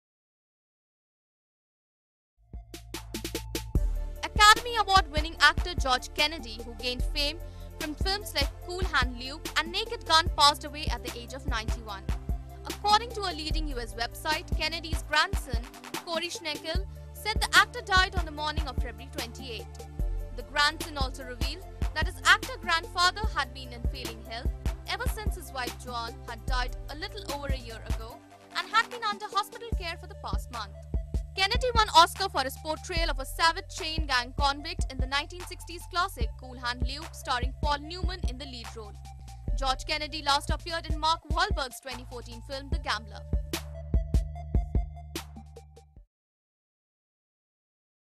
Oscar winning star George Kennedy passes away at 91
Academy Award winning actor George Kennedy, who gained fame from films like 'Cool Hand Luke' and 'Naked Gun' passed away at the age of 91.